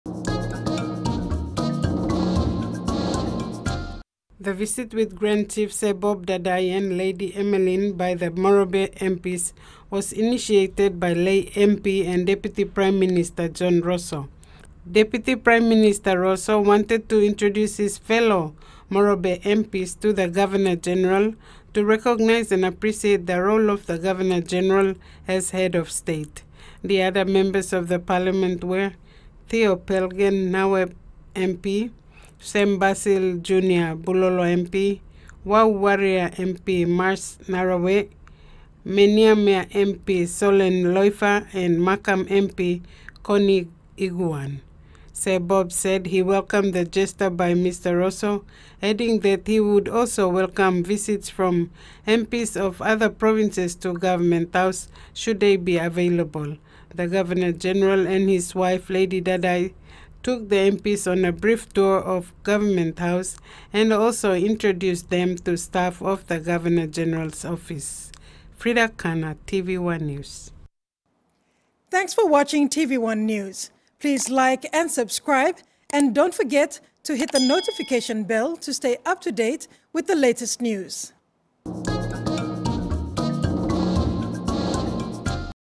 Govt House Visit
Governor General Grand Chief Sir Bob Dadae and Lady Emeline Dadae recently hosted Deputy Prime Minister John Rosso and other members of Parliament from Morobe Province.

The visit took place at the Government House last Friday.